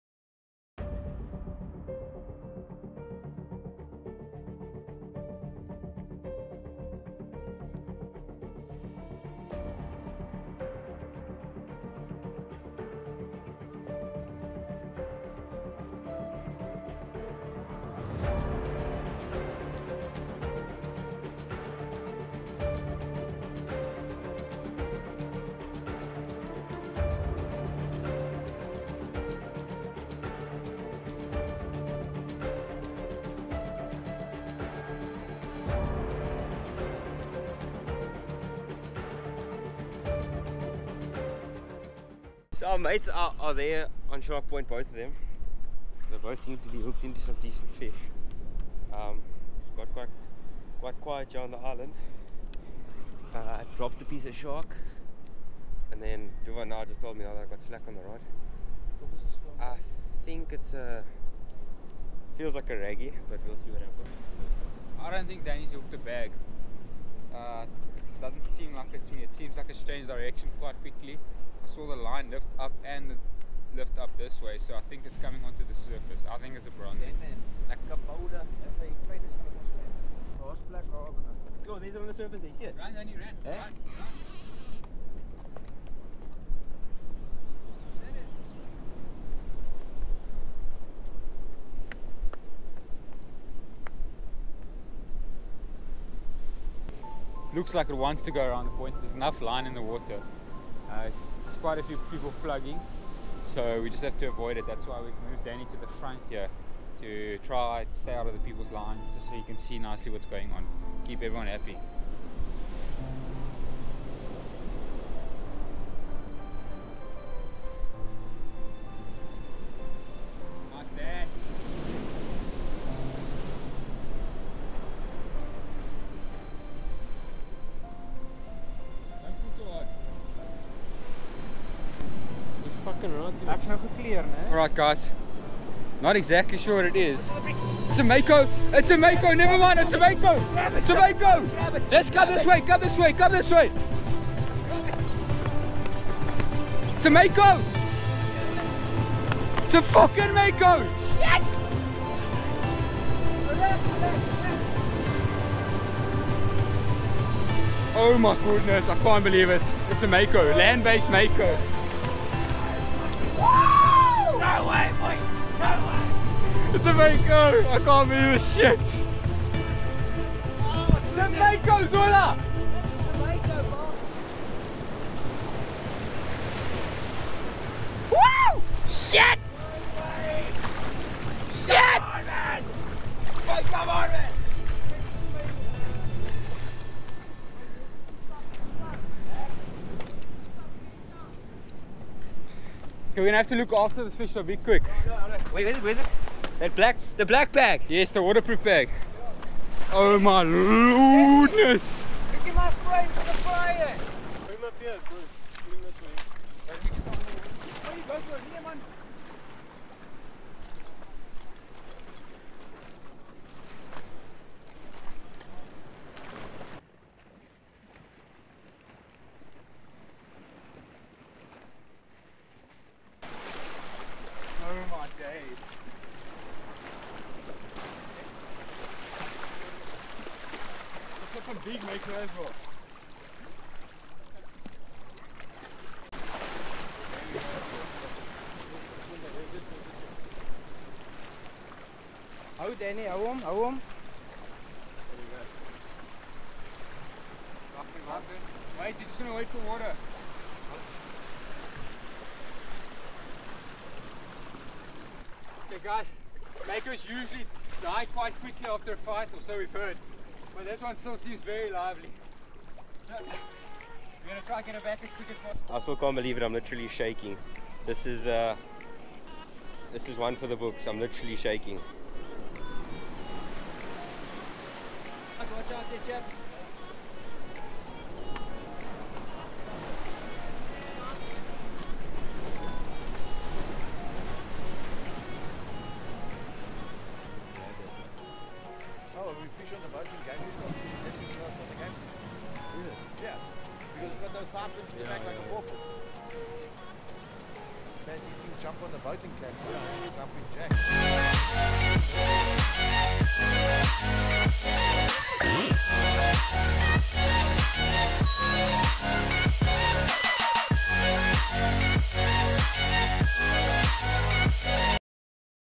FISHERMAN ENCOUNTERS RARE SHARK SPECIES FROM SHORE! || MAKO SHARK
On our recent trip to the amazing Wildcoast, a member of our team managed to catch a mako from shore!
This is extremely rare for any part of our coastline and is pretty much unheard of.

This was going to be part of PT 2 of my Mazeppa series, but I fealt it was unfair for such an amazing fish being just another catch, so I decided to make  its own video!